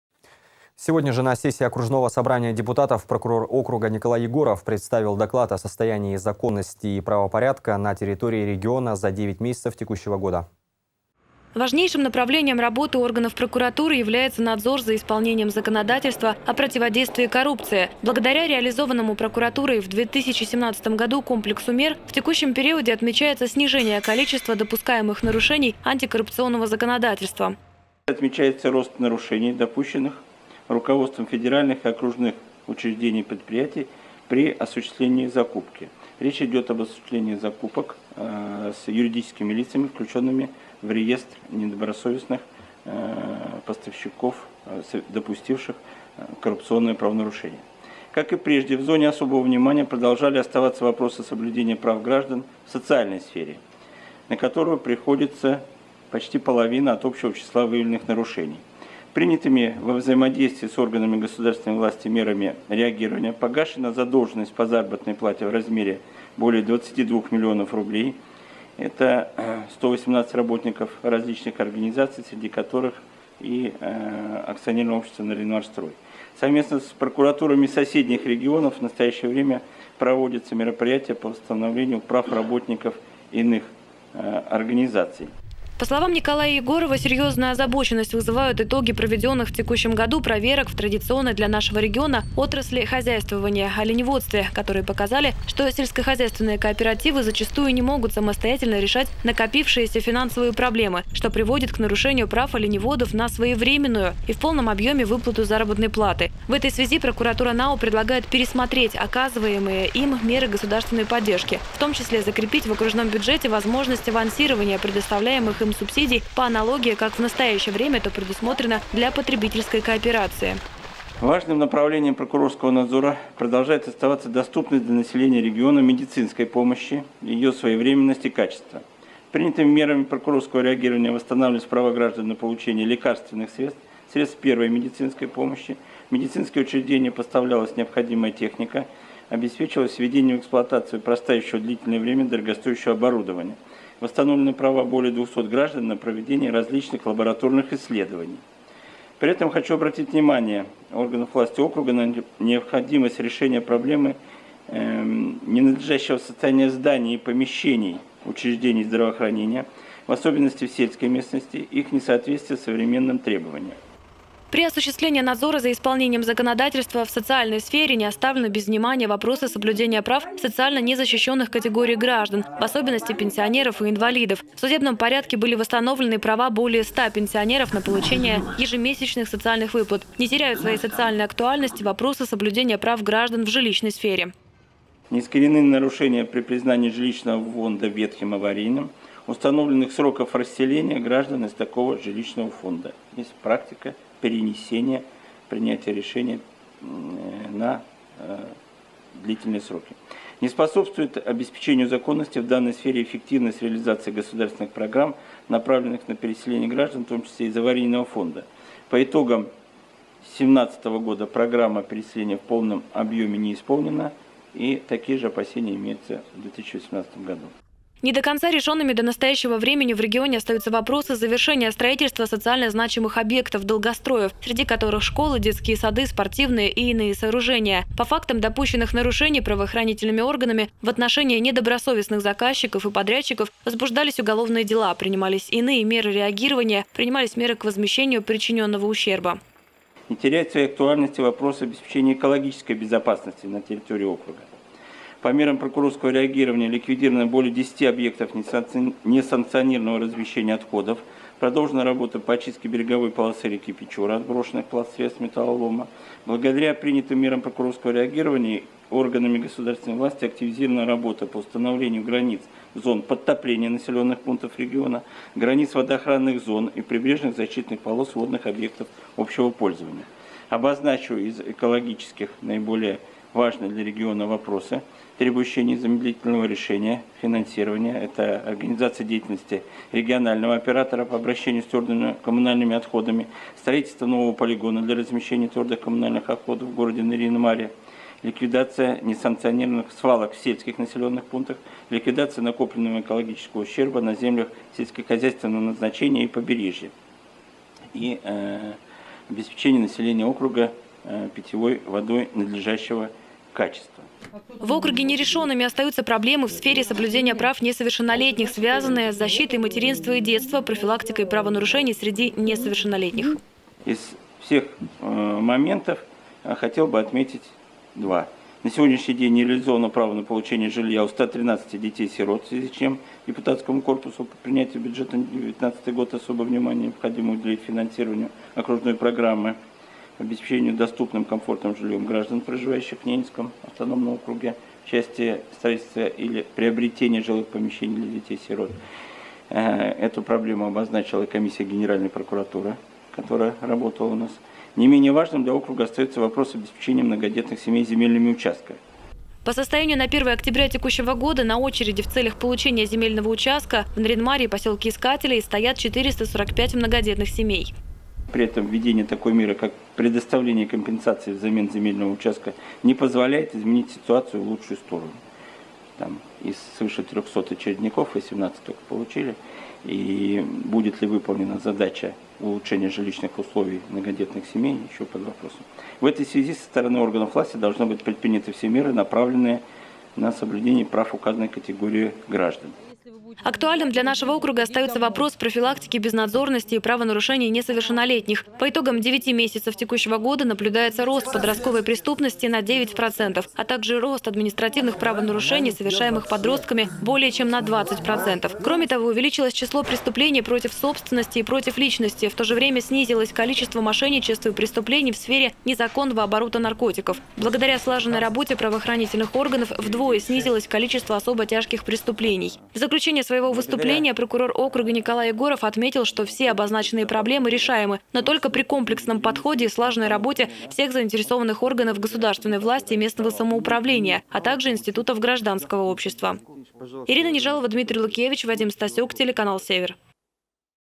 Прокурор Ненецкого округа представил депутатам итоговый доклад
На очередной сессии регионального парламента прокурор округа Николай Егоров представил доклад о состоянии законности и правопорядка на территории региона за 9 месяцев этого года. Руководитель надзорного ведомства озвучил те проблемы, которые, по его мнению, требуют особого внимания и комплексного подхода при их решении.